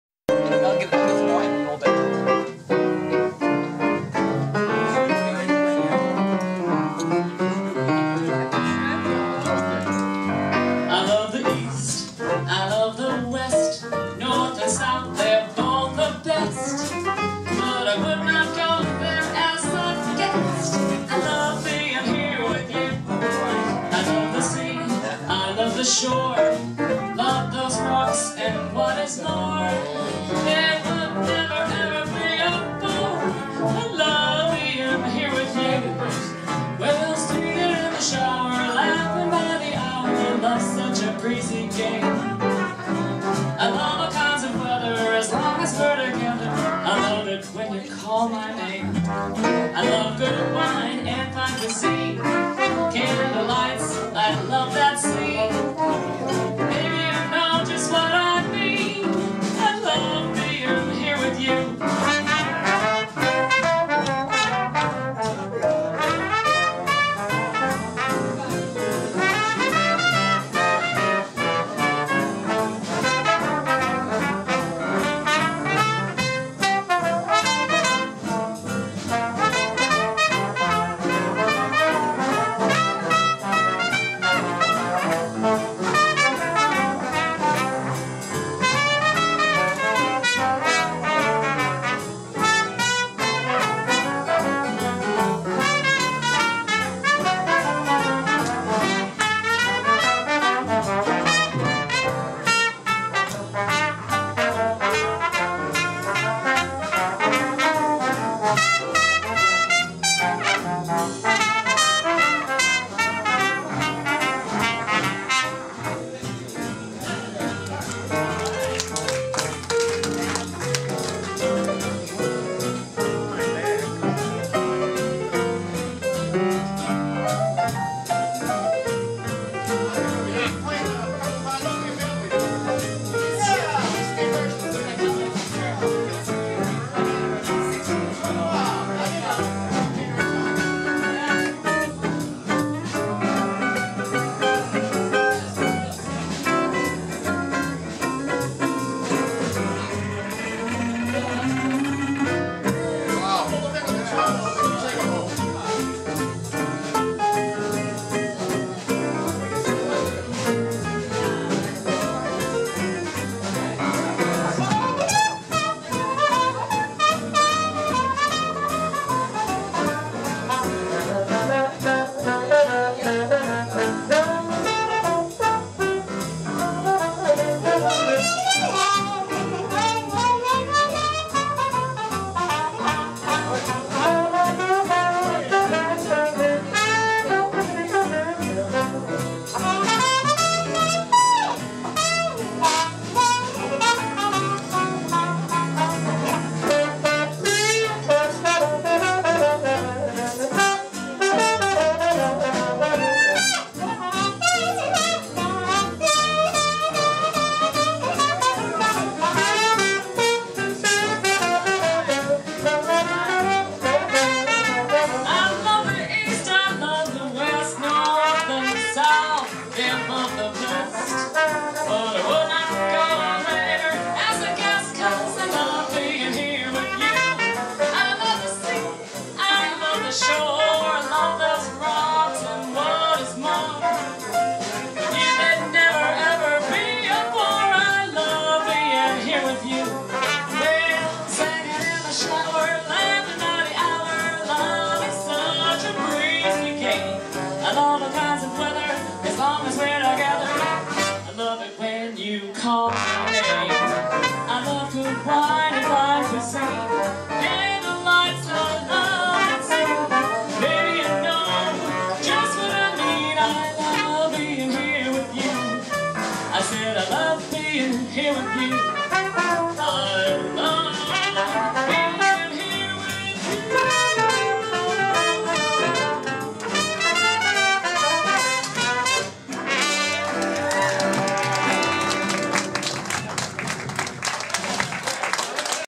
Maureen Loves Being at Acton Jazz with the Seacoast Stompers
Sweet vocal and a great brass duel at the Acton Jazz Cafe

Seacoast Stompers:
Frank Stadler - piano and leader, Dave Whitney - trumpet, Lee Prager - trombone, Scott Philbrick - guitar, Bob McHenry - bass, Bobby Reardon - drums, Maureen Benson - vocal